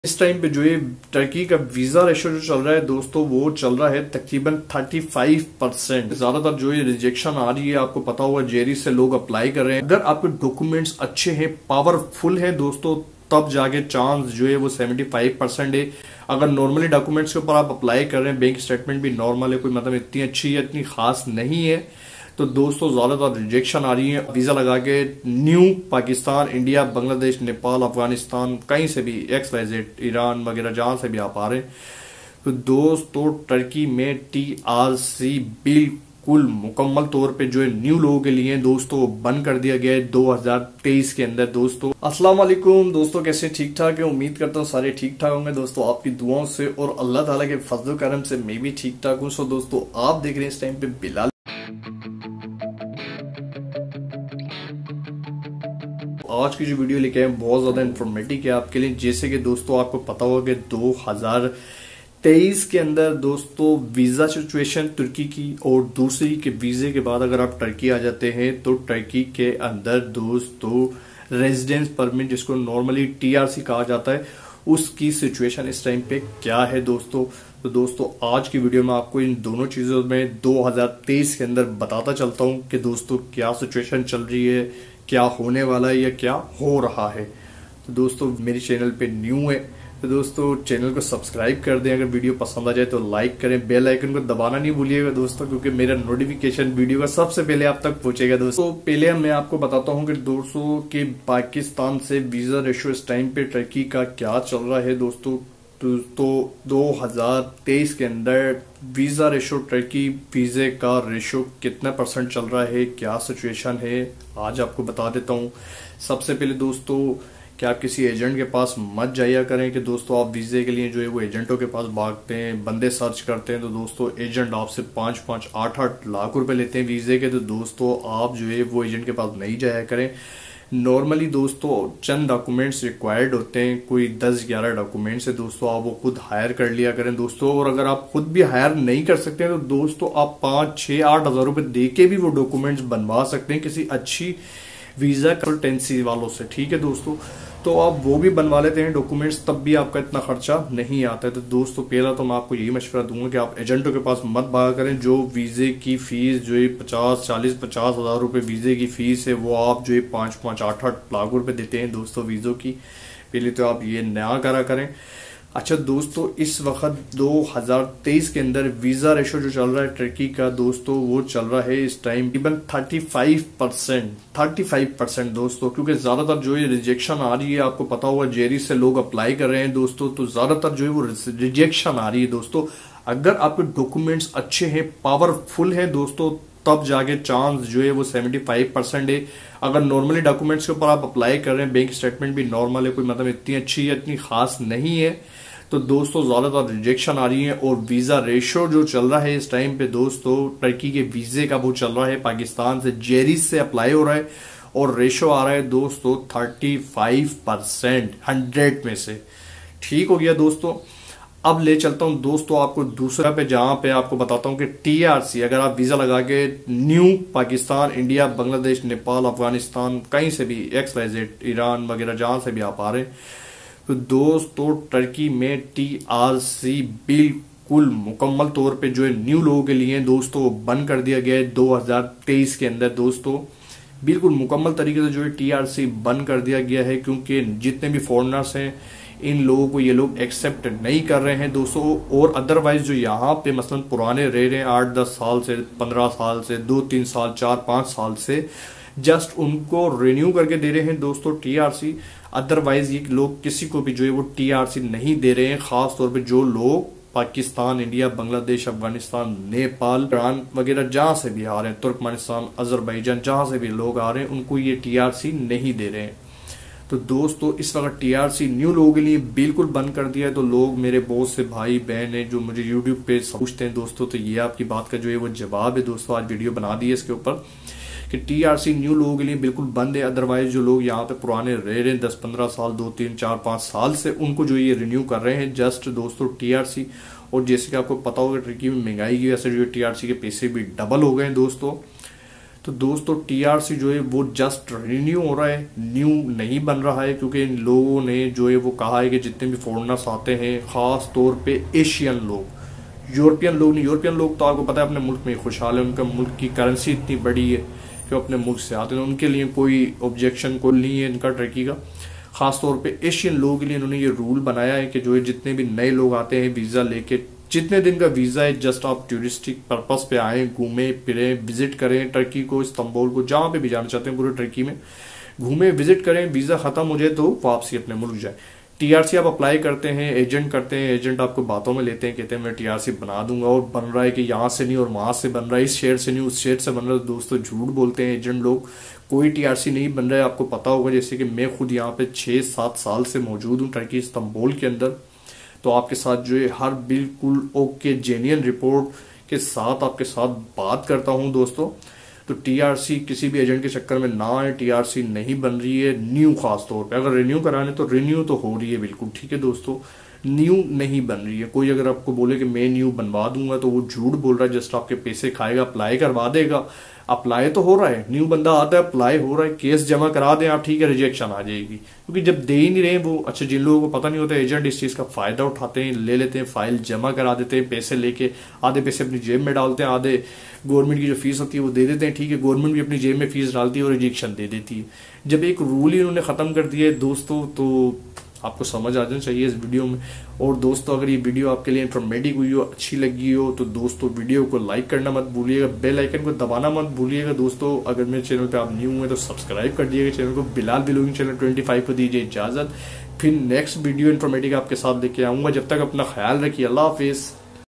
🇹🇷🇵🇰Turkey Visa Ratio 2023 || Turkey TRC Renewal And New TRC update 2023
Friends, you will not see today's video, what is the status of Turkish visas at what time and what percentage of people are getting and how many people are getting rejected and what is the status of the permit in Turkey at this time.